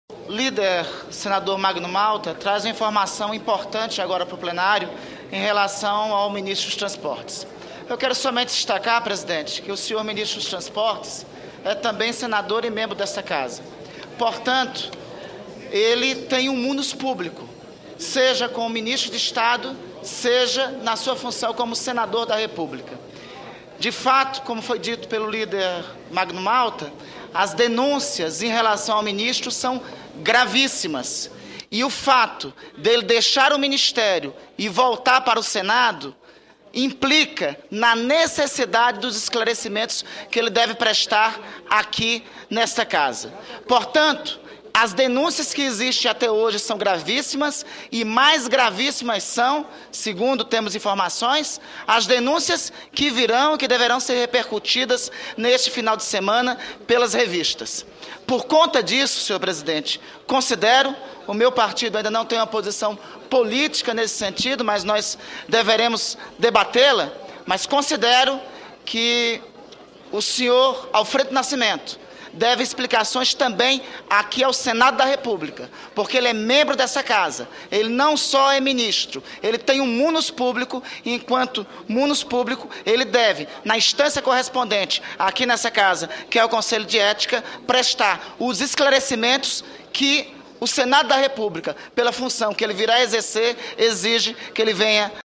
Alfredo Nascimento deve explicações também ao Senado, afirma o sen. Randolfe Rodrigues, PSOL/AP
06/07/2011 - Plenário - Alfredo Nascimento deve explicações também ao Senado, afirma o sen. Randolfe Rodrigues, PSOL/AP